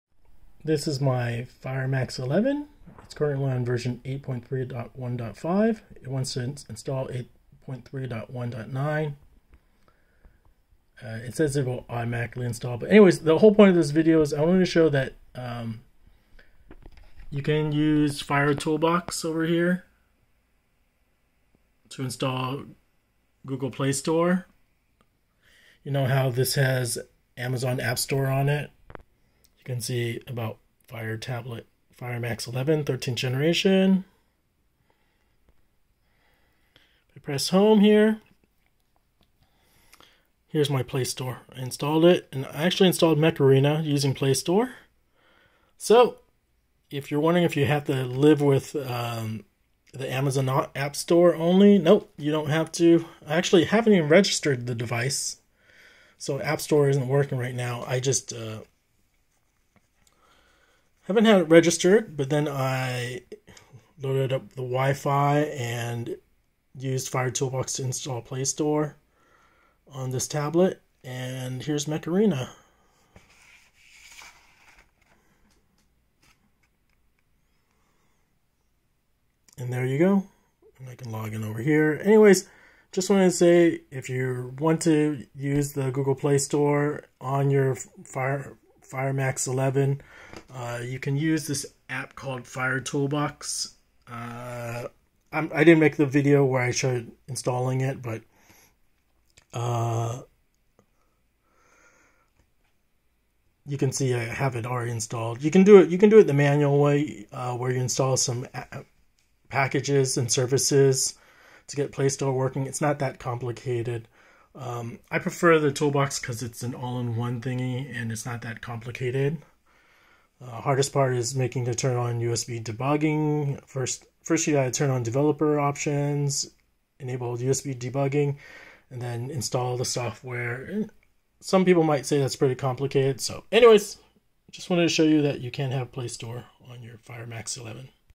I own a Amazon Fire Max 11. I have Google Playstore installed onto it using Fire ToolBox V31 PC
This video is not a tutorial.
This just shows that it's easy and possible to install google playstore to use to install games and apps instead of the amazon appstore.

2.  Pre-setup: Enable Developer options, enable USB Debugging.
3.  Install fire toolbox and Android USB drivers.
4.  Run fire toolbox.. and plug in your max hd 11.  You will get a prompt on your fire hd max if you want to allow the connection.  Click yes.
5.  In fire tool box click on the bottom left option "Google Services"
    It will give you an option to install google playstore.

VOILA.. if everything goes well it will install and ask you to restart your tablet.

That's it.
You can install the google services manually.. it's about 4 package files I believe.. maybe more.  But I think using fire toolbox is easier for me.

My tablet was running Fire OS 8.3.1.5 for march 15 2023.
I used fire toolbox v31 to install google services and playstore.

I hope this eases peoples concerns that there is no google support.  Technically true.. but if you do it yourself.. you can have a google android tablet for cheap.